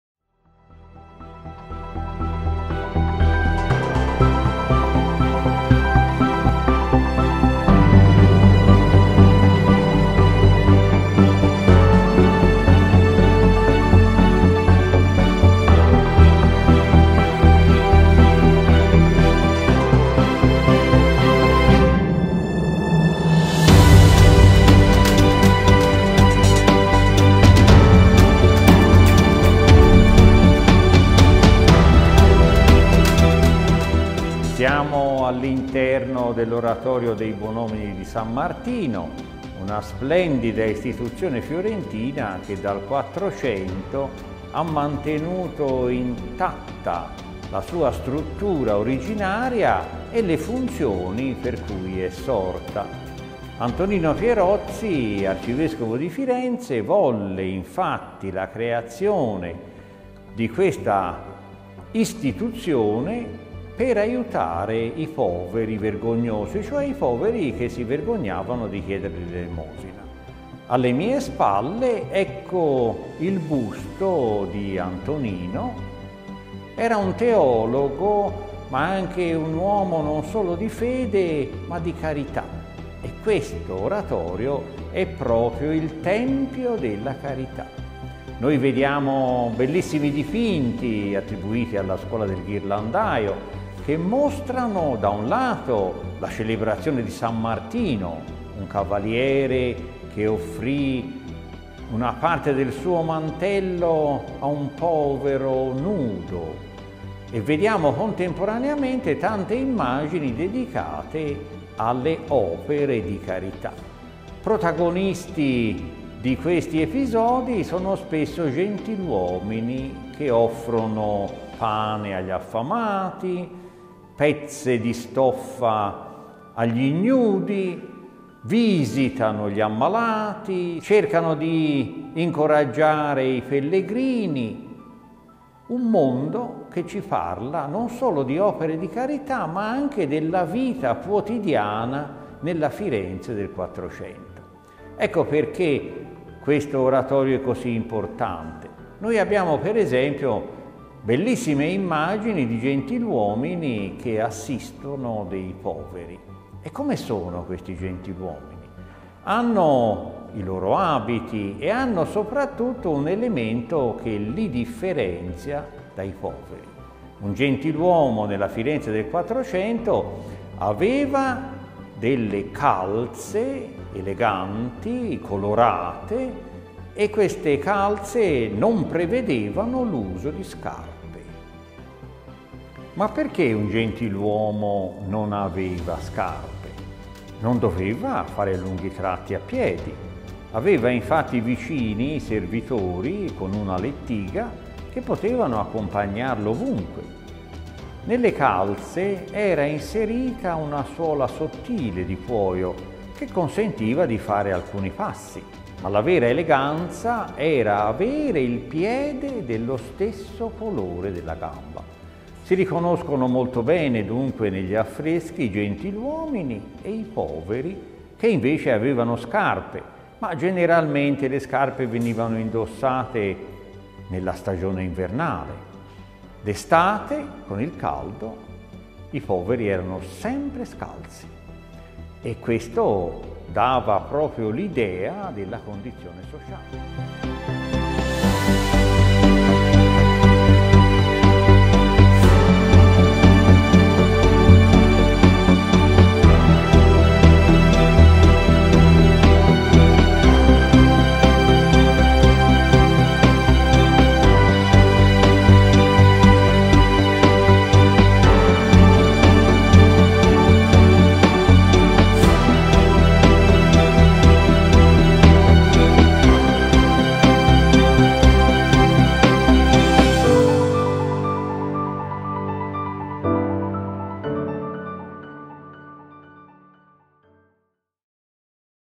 Terme di Firenze - Bellezza, fede e carità
"Gocce di Bellezza tra Petali di Storia",
un viaggio alla scoperta della bellezza attraverso i racconti del Prof. Giovanni Cipriani. In questa puntata si parla della bellezza, la fede e la carità

Luogo: Oratorio dei Buonomini di San Martino, Firenze
Argomento: le opere di carità, la congregazione dei Bonomini di San Martino. Usi e costumi della Firenze del '400.

Si ringraziano:
La Congregazione dei Bonomini di San Martino, fondata a Firenze nel 1442 per volontà di Antonino Pierozzi, Arcivescovo di Firenze.
Il Prof. Giovanni Cipriani, docente di Storia moderna all'Università di Firenze.

Una produzione: achrom.it